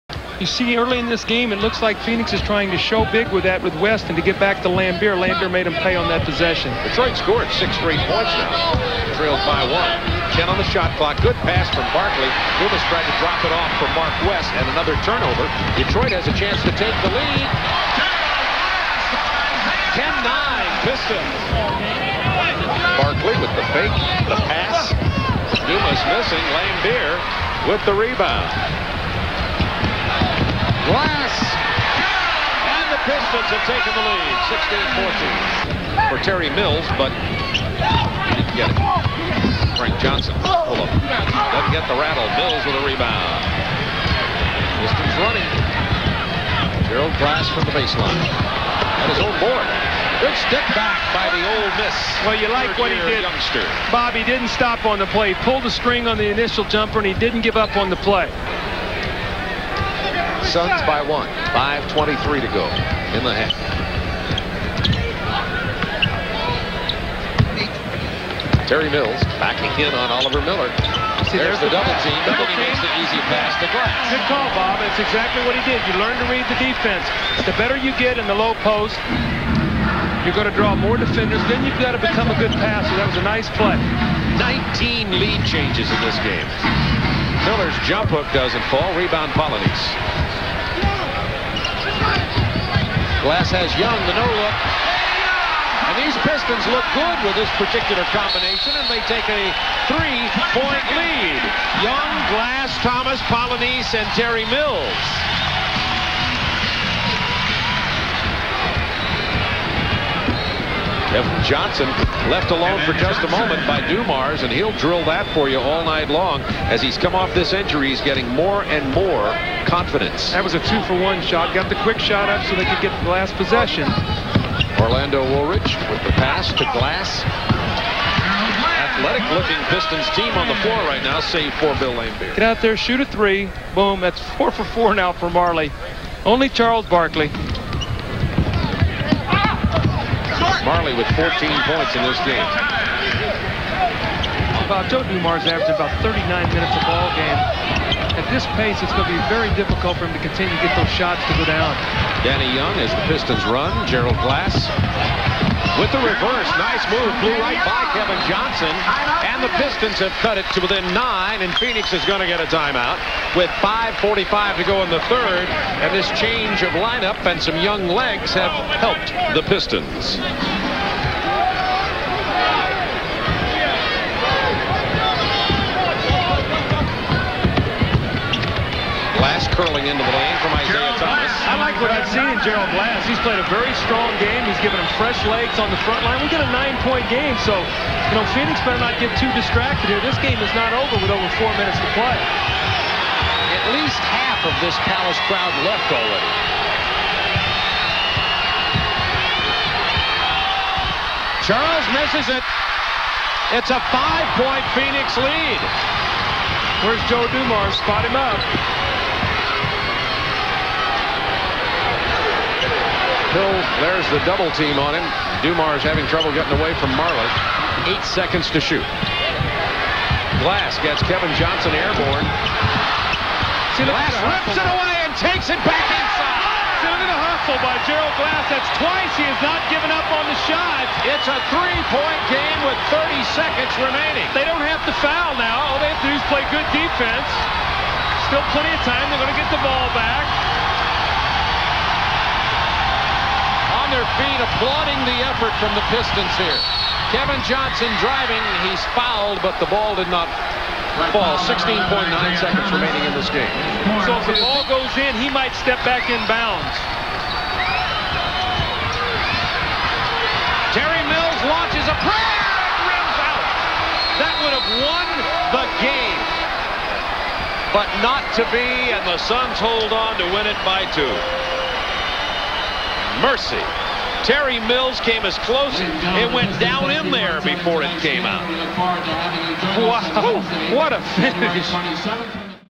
Gerald Glass' Best Game as a Piston: 22 Points, 13 Rebounds, 6 Assists
January 25, 1993 - Sliding into the starting lineup in place of an injured Dennis Rodman, 25-year-old Gerald "World Class" Glass impressed, finishing with 22 points, 13 rebounds, and 6 assists in a narrow 121-119 loss to the eventual NBA finalist Phoenix Suns.

Glass topped out as a 28-point-per-game scorer at Ole Miss, peaking with a 53-point performance against Mahmoud Abdul-Rauf in a 113-112 win over LSU in 1989. Glass parlayed his scoring prowess into becoming a first round draft pick of the Minnesota Timberwolves in 1990.

Struggling to find a consistent role in Minnesota, Glass was dealt to the Detroit Pistons in 1992. Primarily coming off the bench, Glass averaged 5.3 points per game in his lone season in Detroit. It would be his final full season in the NBA. Glass spent the remainder of his professional career bopping around the CBA and Italy before a couple more cups of tea with the New Jersey Nets and Charlotte Hornets.

Among Pistons fans (and dunk fans), Glass may be best remembered for being the recipient of Isiah Thomas' bounce-pass alley-oop